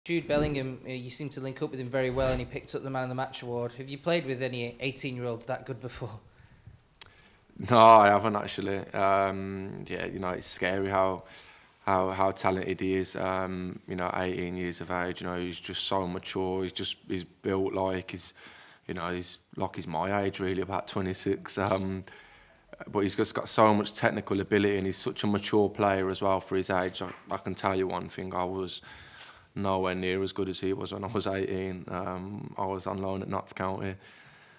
"I was no where near as good as him at 18!" 😅 Jack Grealish praises Jude Bellingham
Jack Grealish praises 18 year-old star Jude Bellingham after a MOTM performance in last night's 3-0 victory over Côte d"Ivoire.